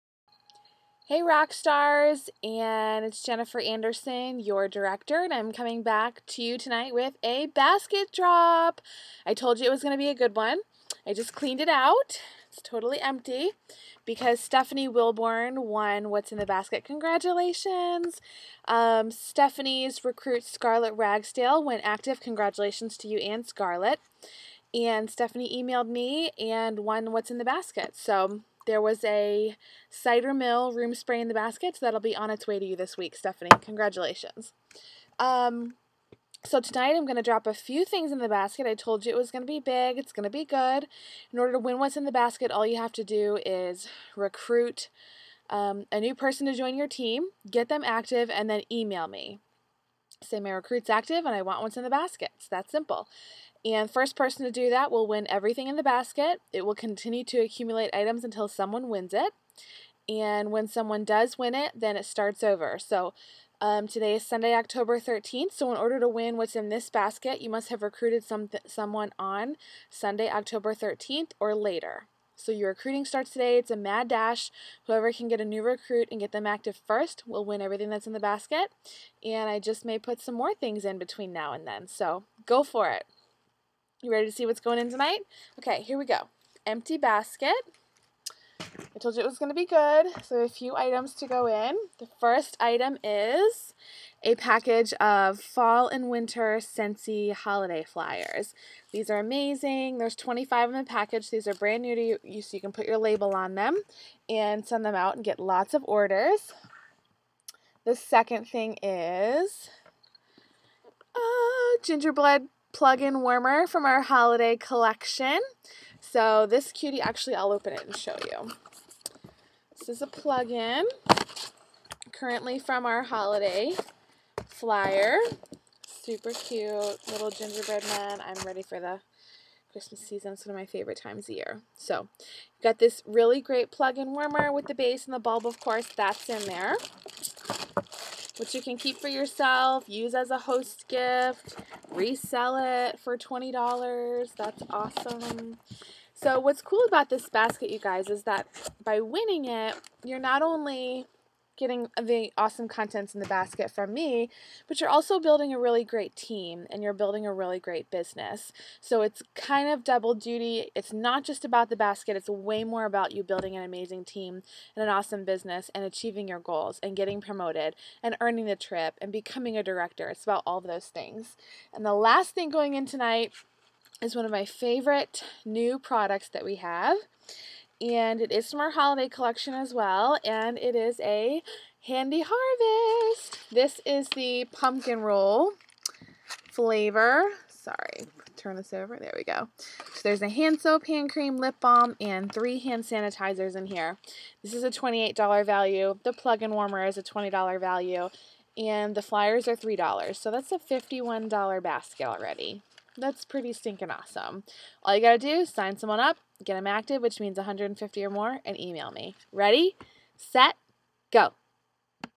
Sweet Basket Drop!
Jennifer Anderson, Director of Radiant RockStars, drops some pretty awesome prizes into the Basket for her Recruiting Incentive. To win everything in the Basket, just Sponsor someone new and get them active! Be a part of these incentives and JOIN HER TEAM